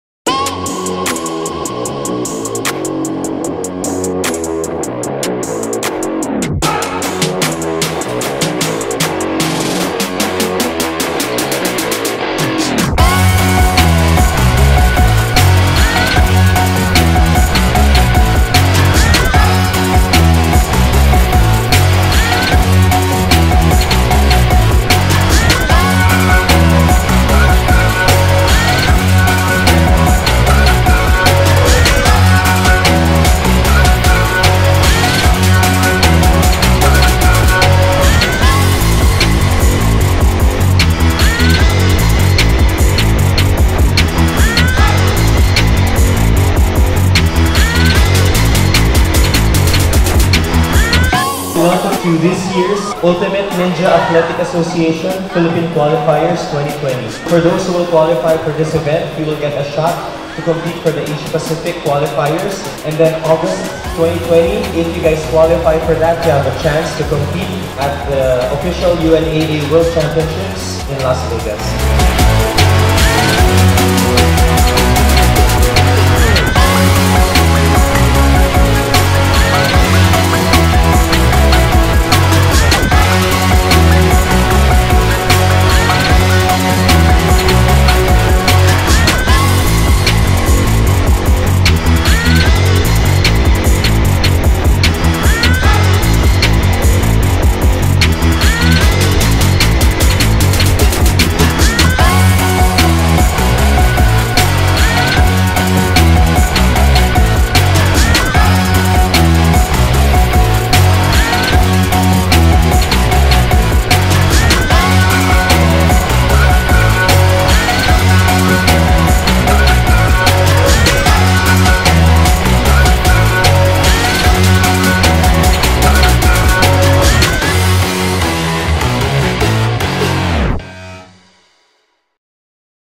Ultimate Ninja Athlete Association Qualifier - Taguig, Philippines // Pretty Huge Obstacles
- Filmed the UNAA Qualifier at Pretty Huge Obstacles in Taguig, Philippines. Great event, looking forward to see who makes the cut for LAS VEGAS this year. Definitely going to be a fun one to watch!

- Instagram, @ocrtube